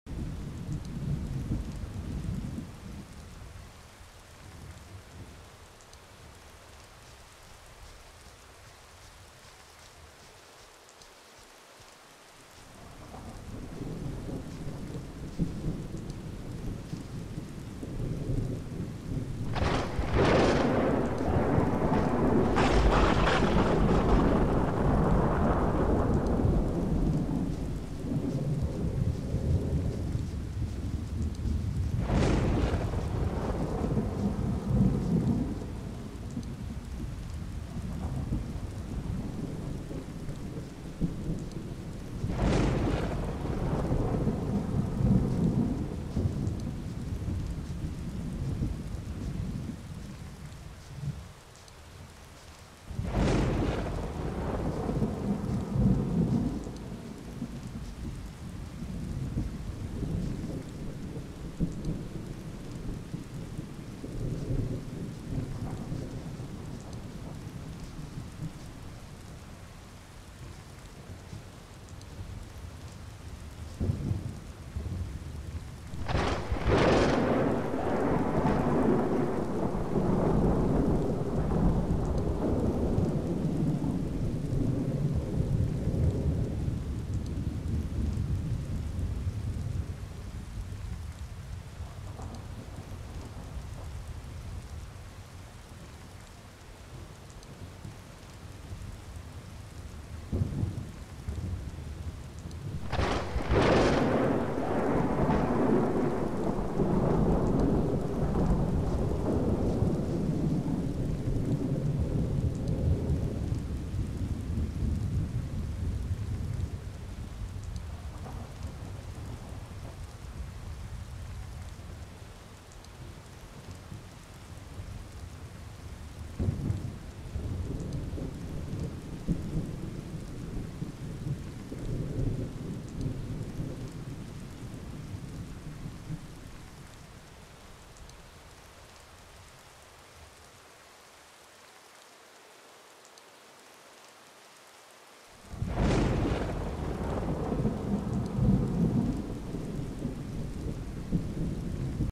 Game Guru - Thunder and Lightning
This is a video demonstration of Thunder and Lightning simulated in the Game Guru Classic engine. Available for purchase on the TGStore